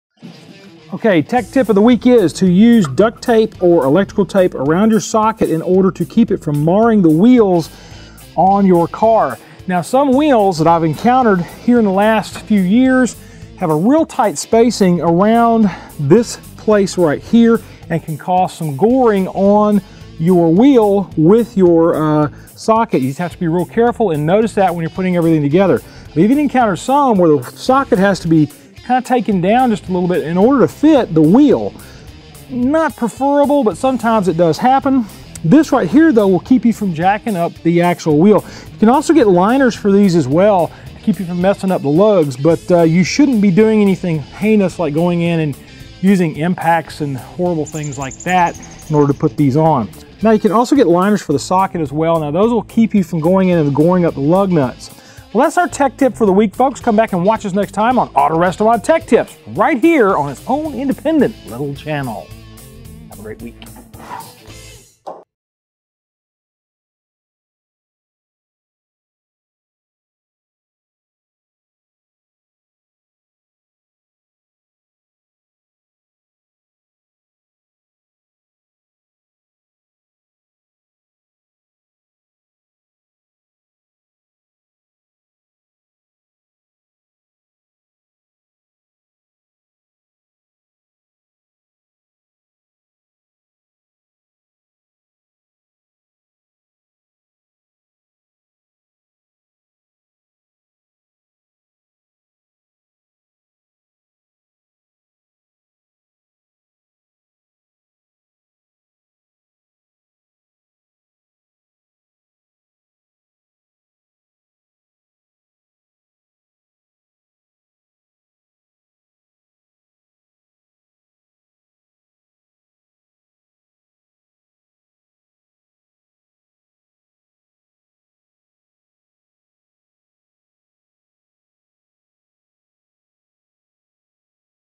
Use Tape to Protect Your Wheels When Taking Off Lug Nuts
Tape your socket to protect the painted surfaces  on your new wheels. You can also get socket protectors to protect the actual lug nuts from damage.